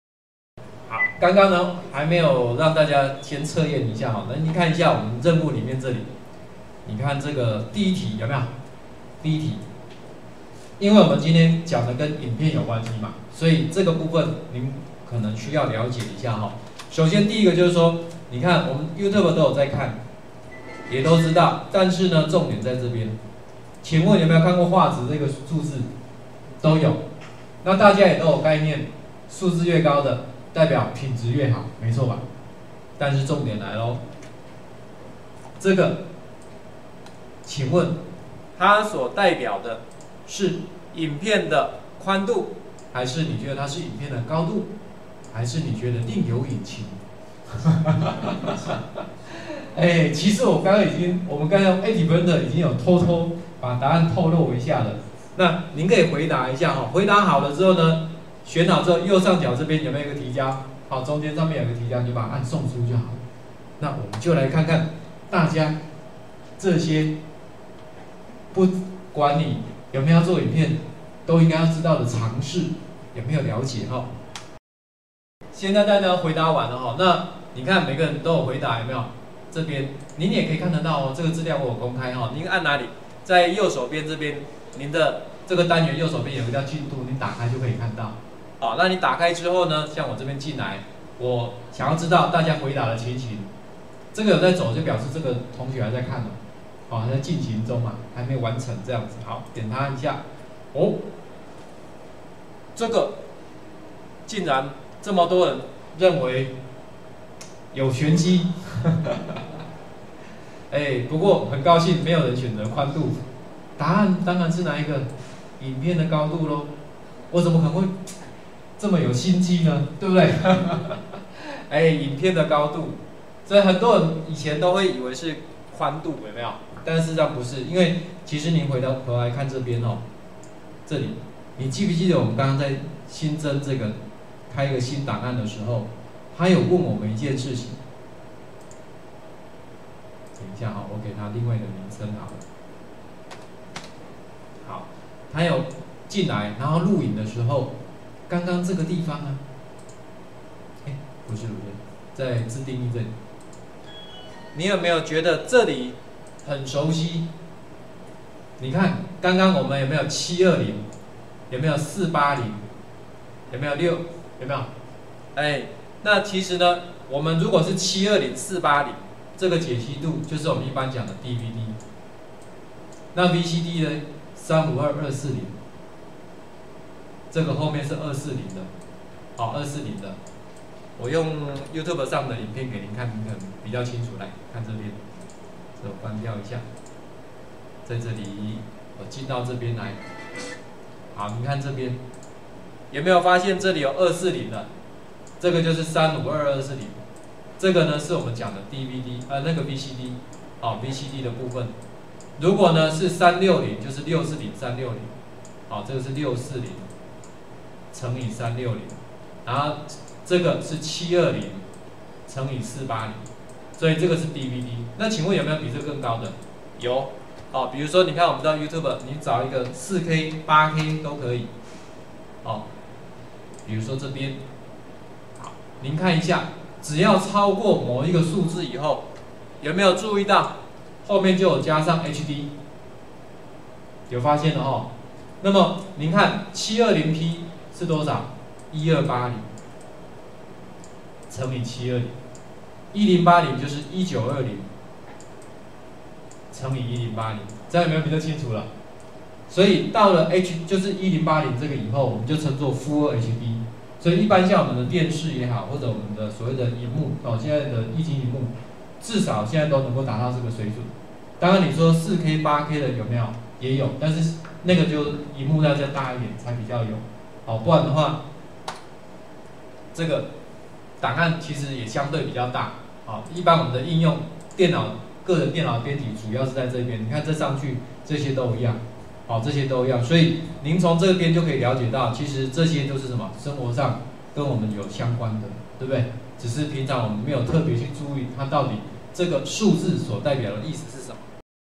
03：Youtube影片品質數字測驗
此研習錄影影片為「20161012虎尾科大：原來數位教材編製與錄影 一個人就能輕鬆完成(2016)」的研習錄影！更多詳細訊息請參閱：述文老師部落格

最近國內外大學興起 MOOCs：Massive Open Online Courses 大規模免費線上開放式課程，臺灣稱做「磨課師」，大陸稱做「幕課」，以 5 至 10 分鐘單元的影片課程，配合即時線上討論與回饋、線上同儕合作學習與討論、線上練習與評量…等，讓學生可以依自己學習的速度安排學習進度與重複練習！

老師們想要達成 MOOCs 教學目標前，必須先從如何進行教材影片製作與教學現場的錄製開始！

研習中主要介紹 ActivePresenter 與威力導演這兩套軟體為主，前者在影片剪輯部份是完全免費，也具有中文界面(內建簡體中文，免安裝版有將繁體中文語系加入)！威力導演除了有些學校已經有購買校園授權版本外，官方也有提供免費特別版(目前是13.0)，約可使用標準版六成左右的功能，對於一般的影片剪輯、藍綠幕去背，都沒有問題！